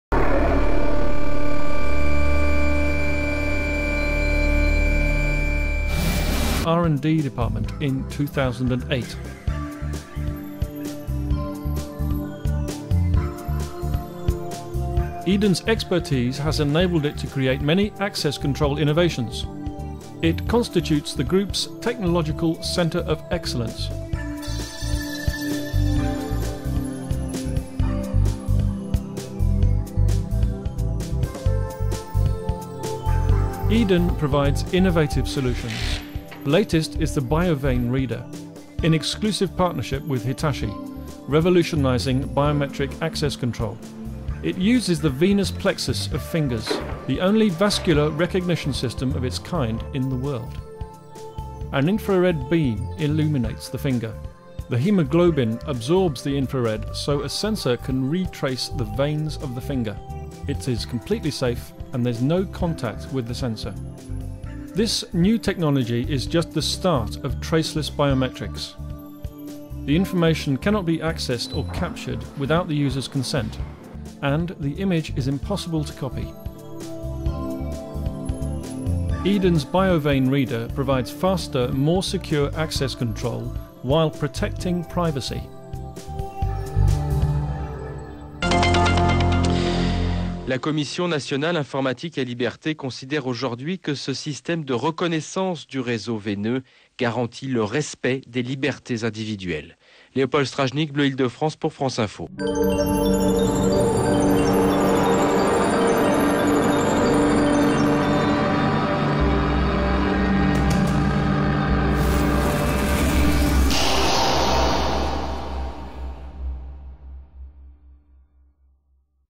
Biovein rewolucyjny czytnik EDEN, biometria żył dzięki sensorowi HITACHI
ControlSYS oficjalny polski dystrybutor produktów Eden. BioVein jest rewolucyjnym urządzeniem kontroli dostępu.Rozpoznaje jedyną niepodrabialną sygnaturę biometryczną znaną dzisiaj: sieć naczyń krwionośnych palca. Zastosowanie sensora firmy Hitachi zapewnia najwyższy poziom bezpieczeństwa, czego efektem jest zastosowanie w 80% banków w Japonii.